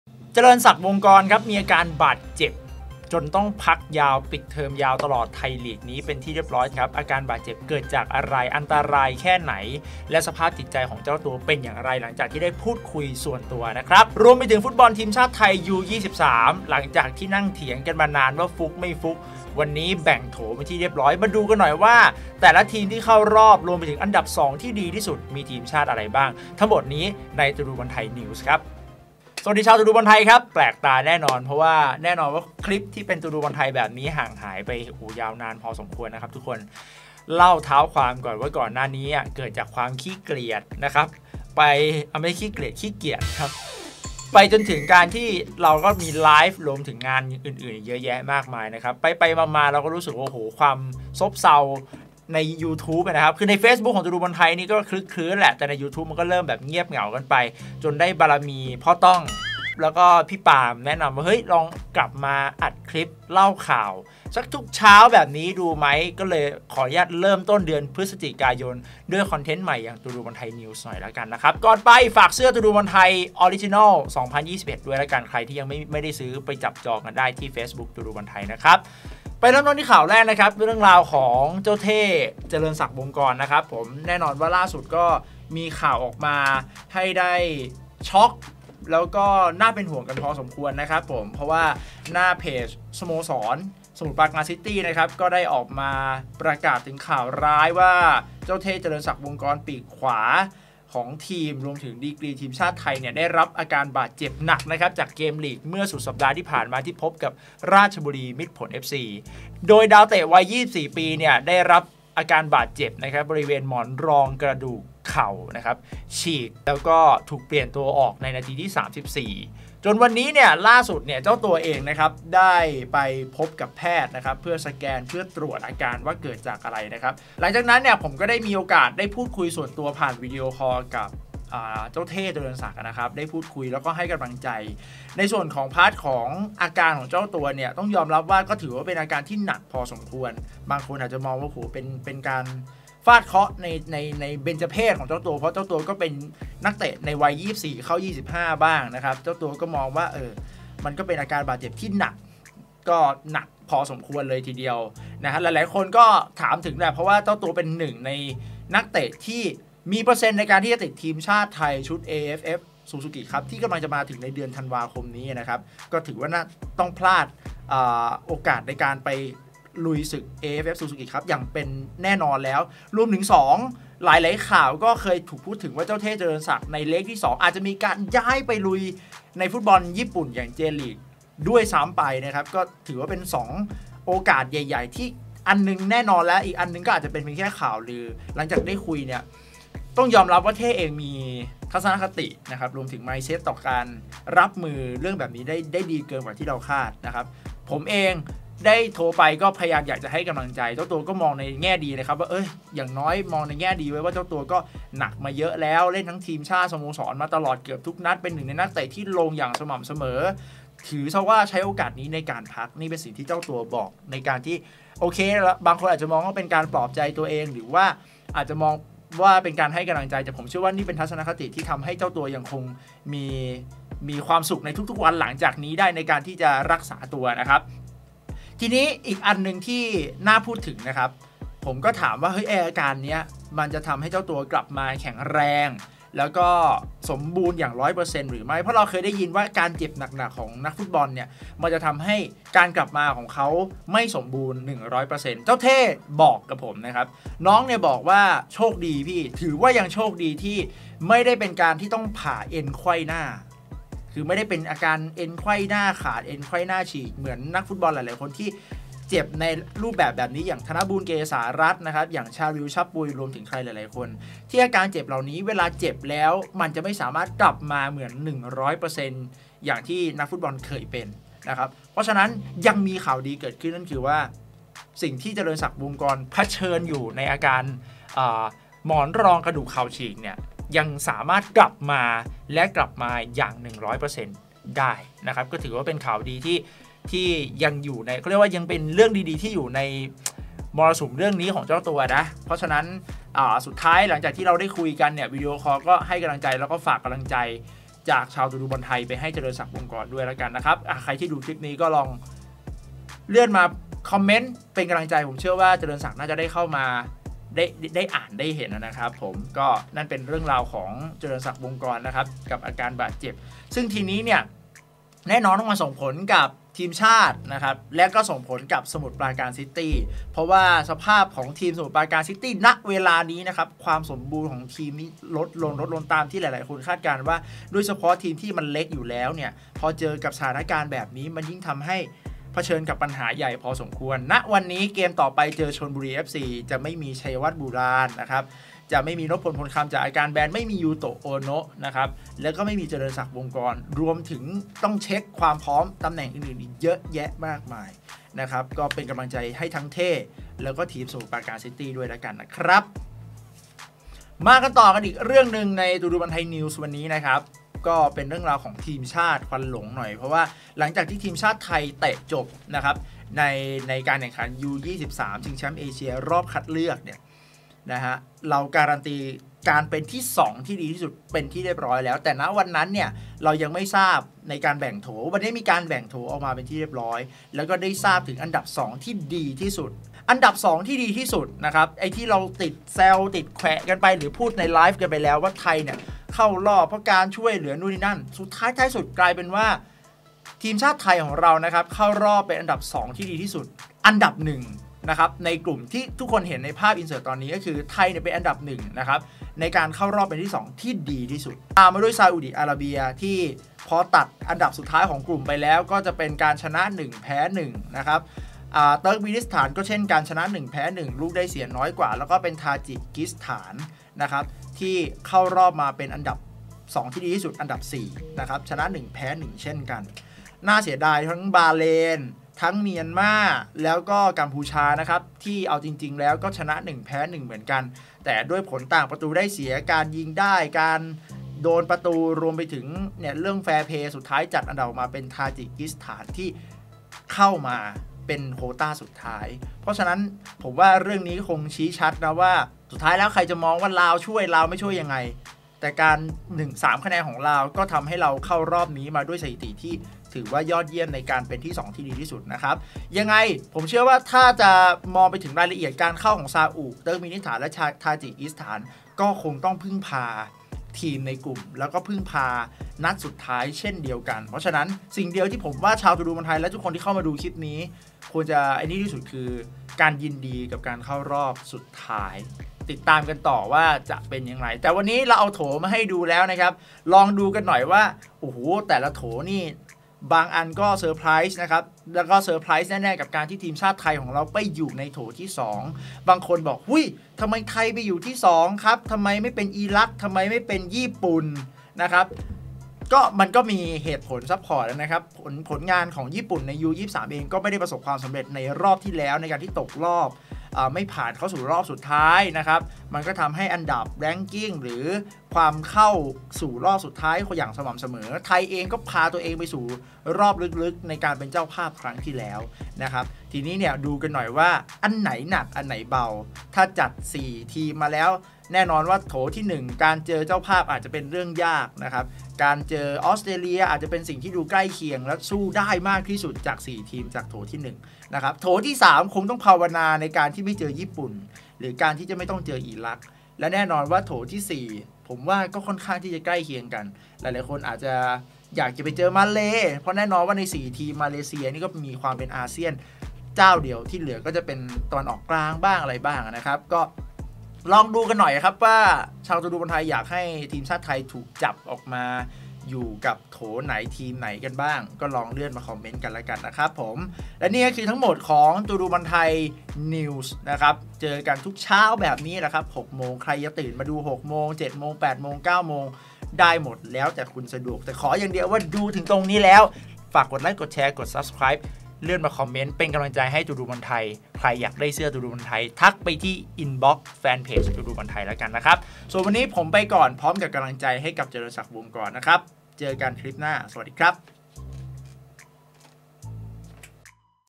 เจริญศักดิ์ เจ็บยาว!! หนักแค่ไหน/ U23ไทยแบ่งโถแล้ว ลุ้นเจอใคร? | ตูดูบอลไทย News
⚽ Ballthai.com : เว็บข่าวสารฟุตบอลไทยอันดับหนึ่ง
⚽ รักกีฬา เชียร์กีฬา งดเหล้า เลี่ยงพนัน โดย มูลนิธิรณรงค์หยุดพนัน